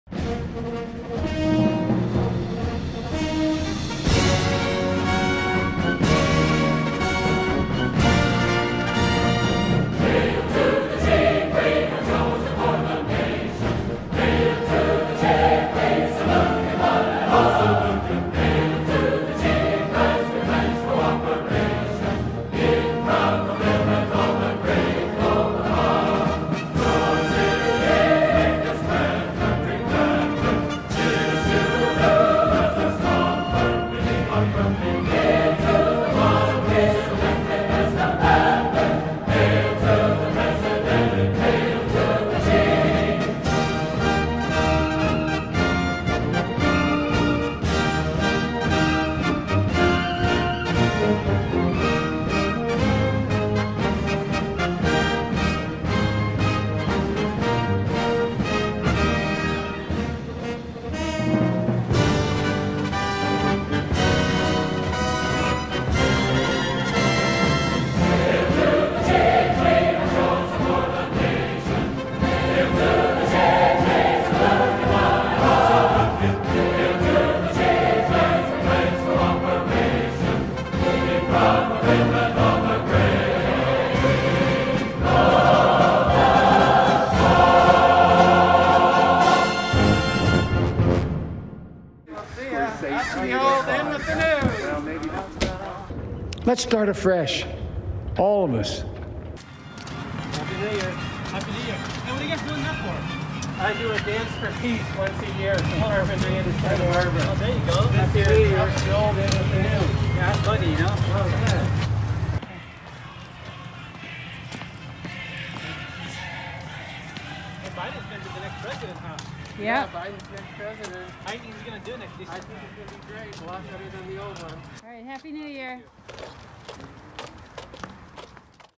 Dance for Peace 2021 by Larry Nimmer
Here's my Dance for Peace 2021.  "Out with the old, in with the new" featuring Trump and Biden.  Larry Nimmer